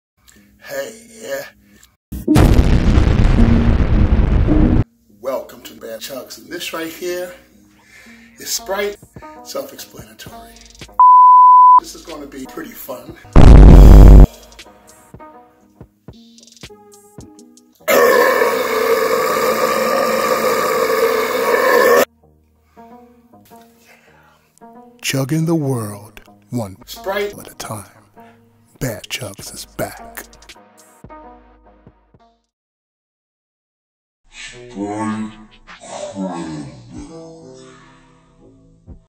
[YTP] BADLANDS SIPS SPRITE
Eric “Badlands” Booker (born June 21, 1969), known professionally as Badlands Booker or BadlandsChugs, is an American competitive eater, rapper and YouTube personality.